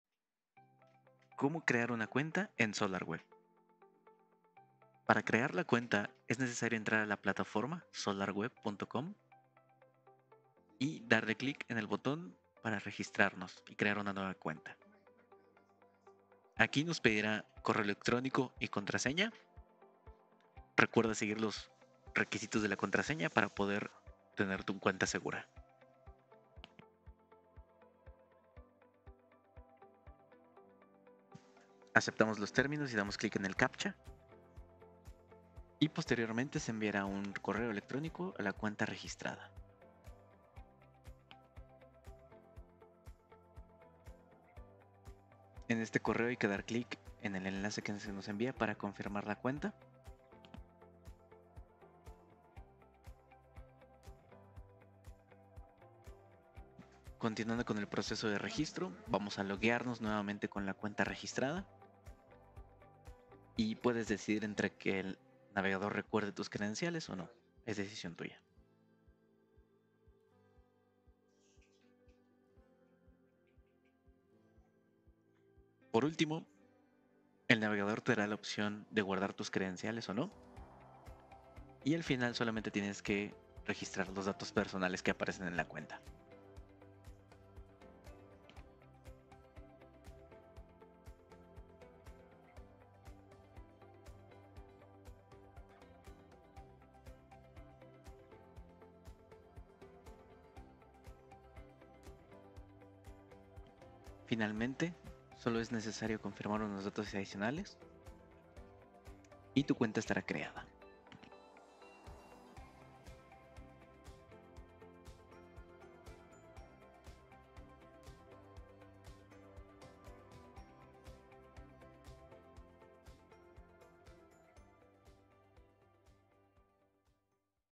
How-To Video: Como crear una cuenta en solar.web (MX)
¡Bienvenidos a otro How-To Video! En esta ocación, te mostramos cómo crear una cuenta en la plataforma Solar.Web de Fronius paso a paso. 🌞💻

Consejos:

Utiliza una contraseña segura para proteger tu cuenta.
Revisa la carpeta de spam si no recibes el correo de verificación o desde Solar.Web.